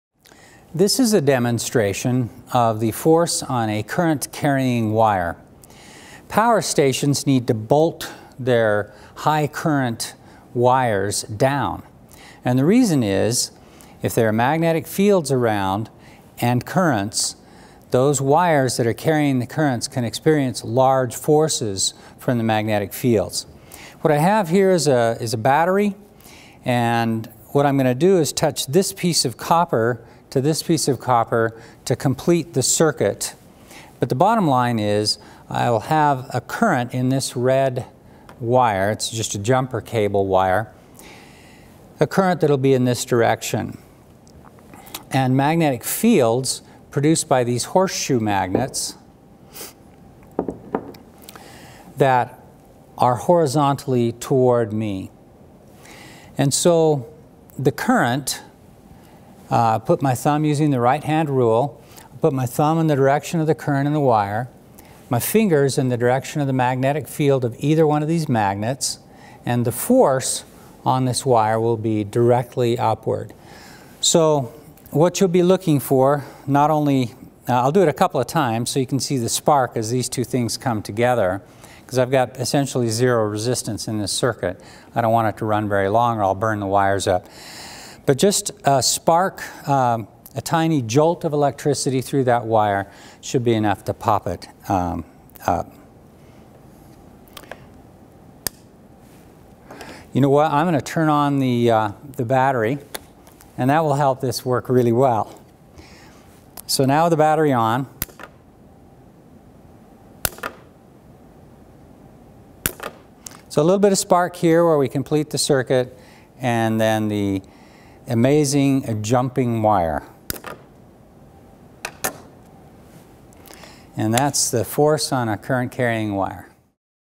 Magnetic Force on a Wire Demo: Jumper Cable
This is a demonstration of the force on a current-carrying wire.  A pulse of direct current is created in a horizontal jumper cable in the presence of a horizontal magnetic field that is perpendicular to the cable, resulting in a vertical magnetic force that causes the cable to jump into the air.